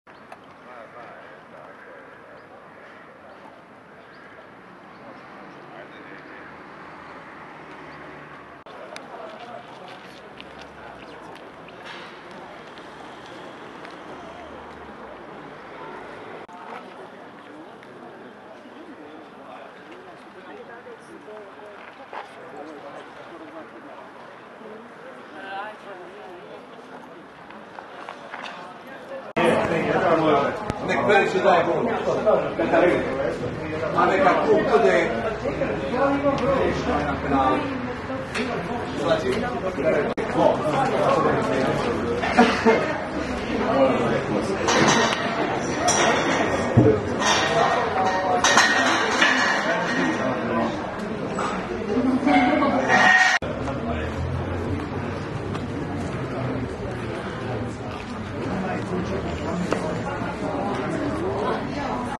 Počela je prodaja karata za derbi između Hajduka i Dinama
U Splitu je počela prodaja ulaznica za dva velika derbija koje Hajduk igra protiv Dinama. Prvi, prvenstveni, na rasporedu je u subotu 30. ožujka, dok se utakmica polufinala SuperSport Hrvatskog nogometnog kupa igra četiri dana kasnije, u srijedu 3. travnja.